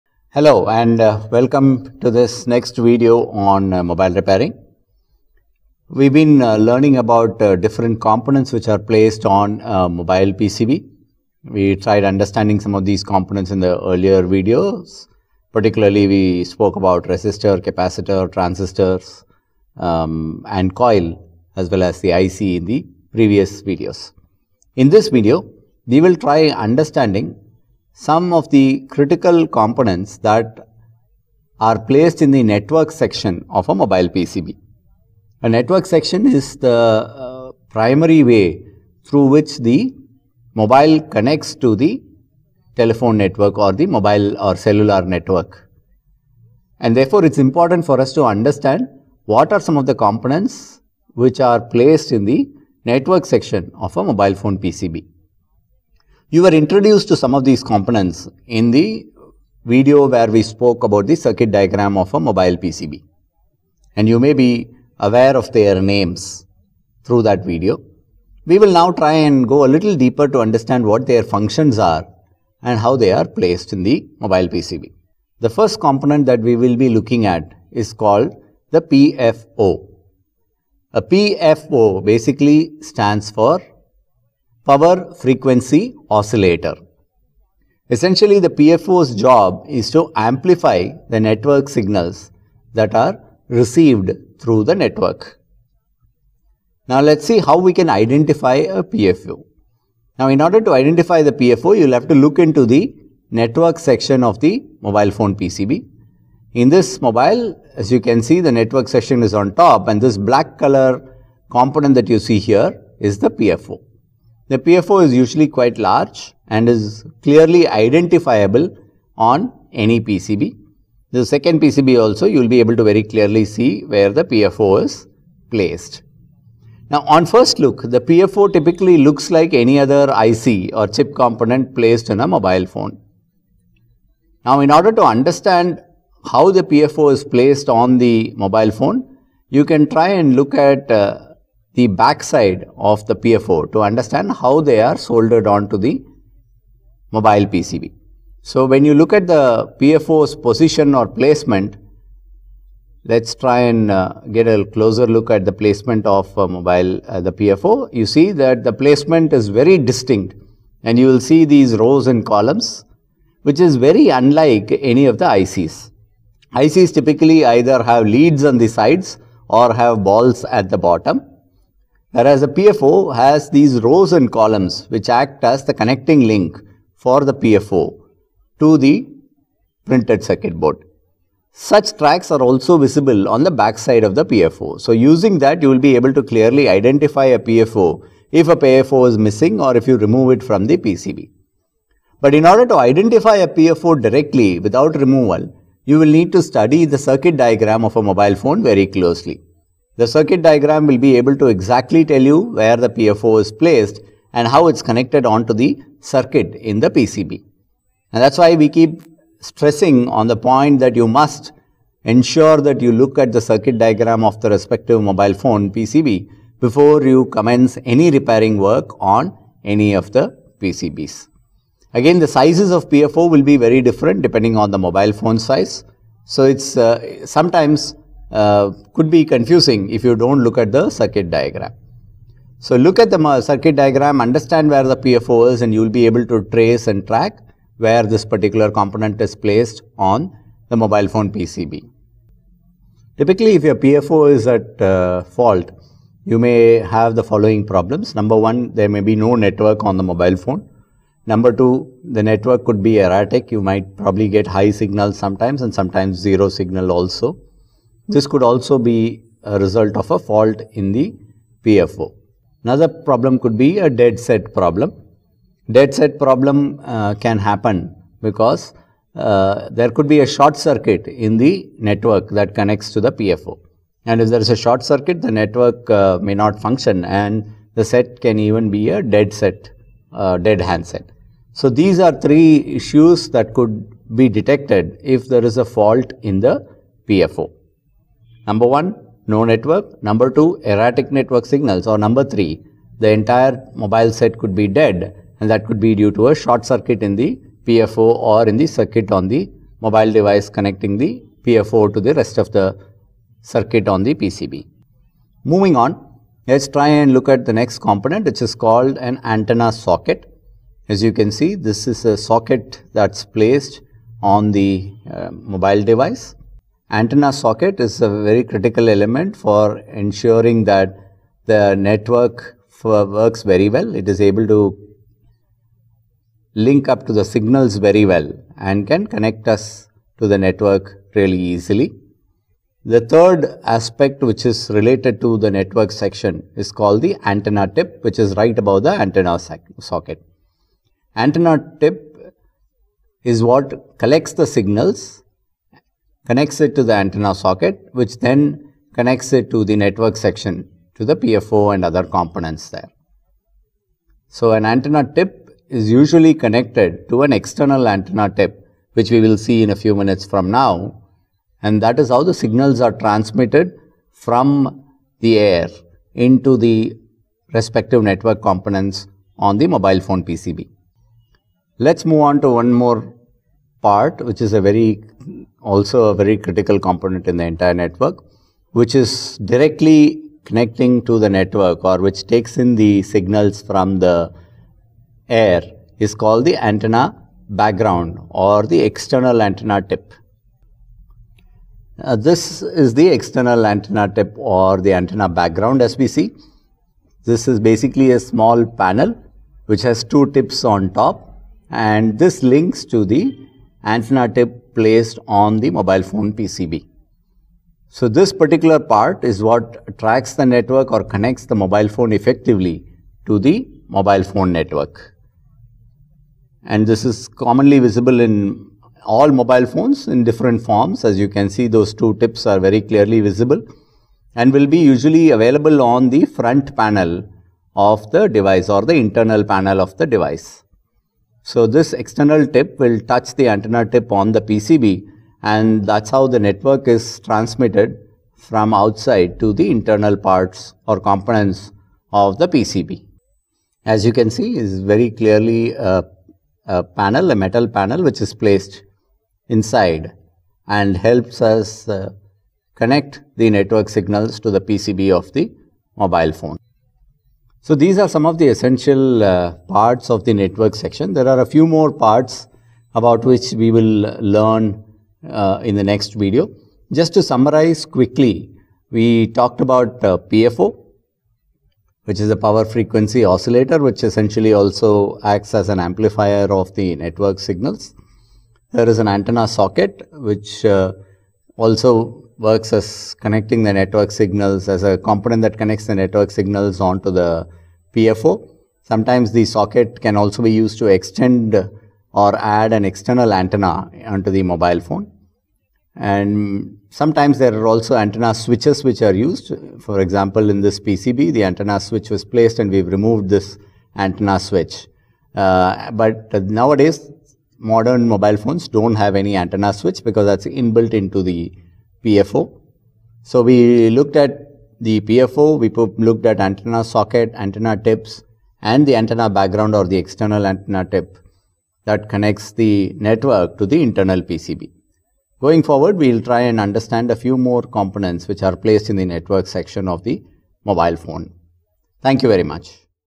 Understanding the Chip Component of a Mobile Phone (Part-1) (English)
In this video you will get get to know more about the Chip Component of the mobile phone.

इस विडियो में आप मोबाइल फोन के चिप भाग से परिचय होंगे।